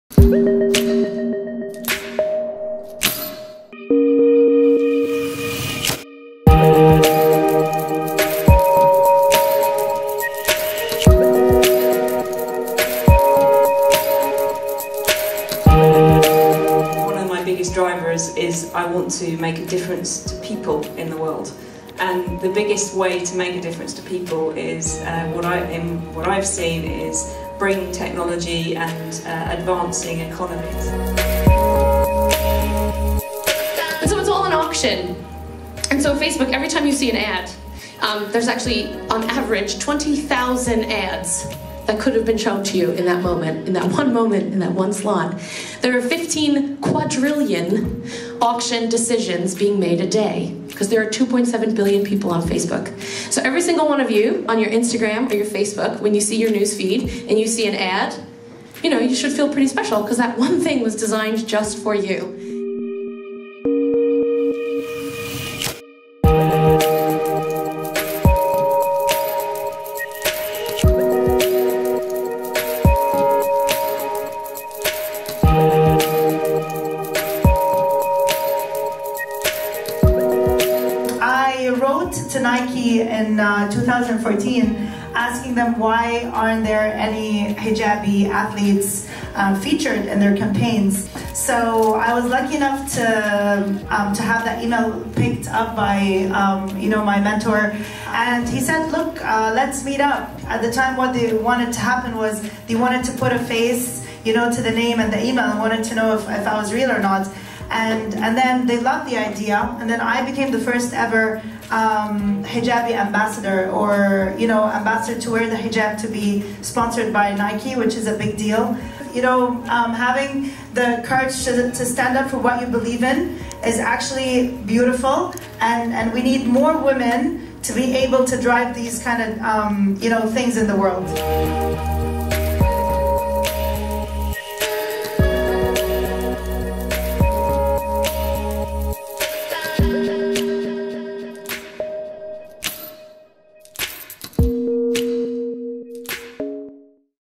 HERFacebook | thinkHERambition x Facebook
14HQ covered the inspiring thinkHERambition event held at one of Facebook's London Offices. The event; aimed at inspiring young females, included different workshops and guest speakers throughout the day.

14HQ provided 2 videographers, 2 photographers and edited this original piece.  For more information on how to book us please head to our website below.